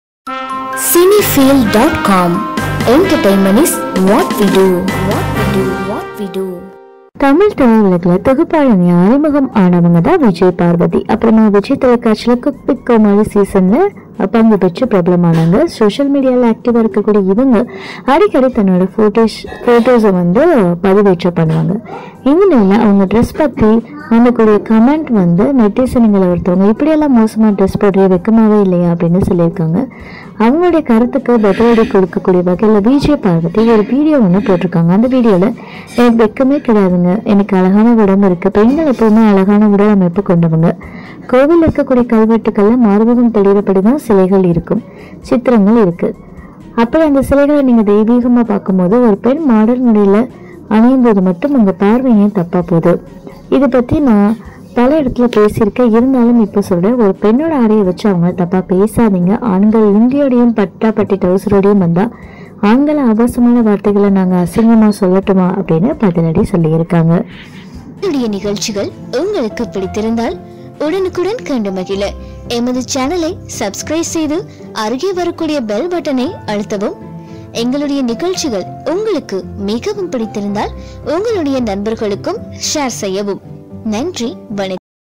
எனக்கு வெட்கமே கிடையாது; அழகான உடம்பு உள்ளது - நெட்டிசனுக்கு பதிலடி கொடுத்த விஜே பார்வதி- Cine Field
எனக்கு வெட்கமே கிடையாது; அழகான உடம்பு உள்ளது - நெட்டிசனுக்கு பதிலடி கொடுத்த விஜே பார்வதி